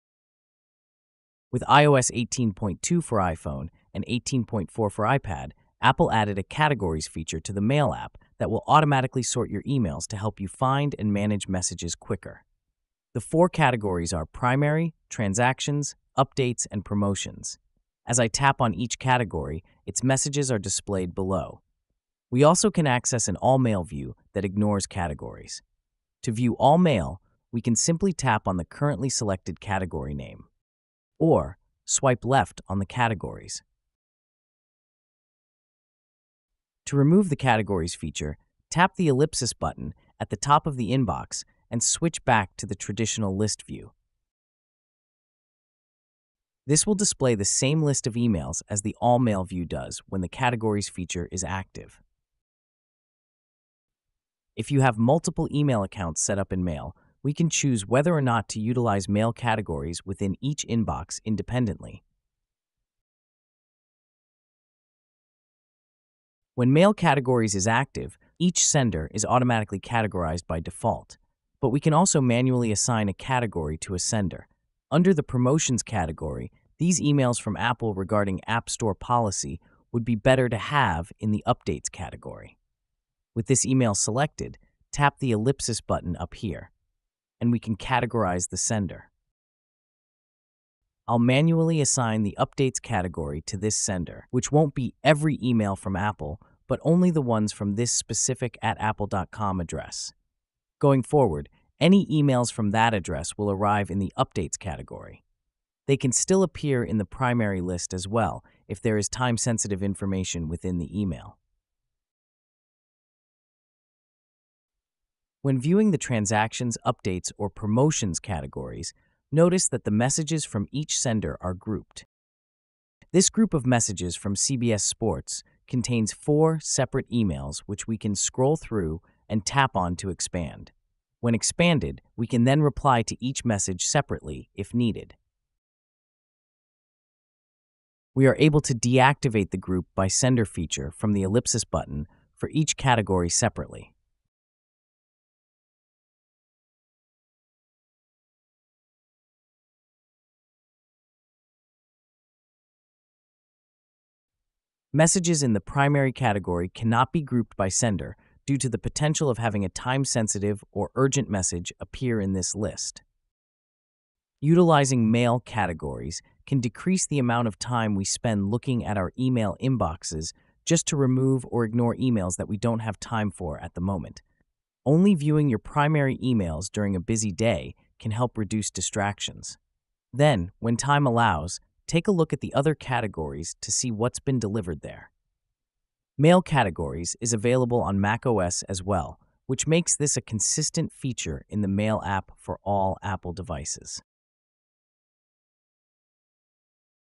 How to use Mail Categories in the Apple Mail App on iPhone & iPad! iOS 18.4 required.
In this lesson that has been added to our full tutorial on Apple Mail for iOS see how to use the new Mail Categories feature. This was added back in iOS 18.2 for iPhone and 18.4 for iPad & Mac. When activated Mail will automatically categorize messages as they arrive. In this video learn how to turn mail categories on or off as well as how to manually assign a category to a specific sender.